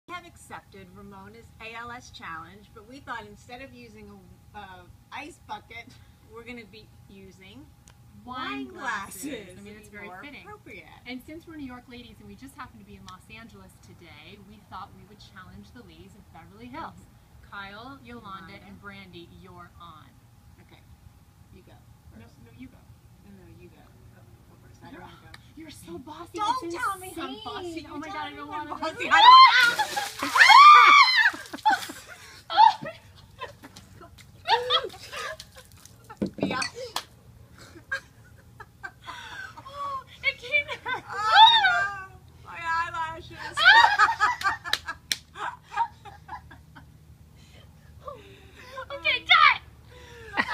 IMG icebucket Wi Fi
RHONY Kristen Taekman and Carole Radziwill take the ALS Ice Bucket Challenge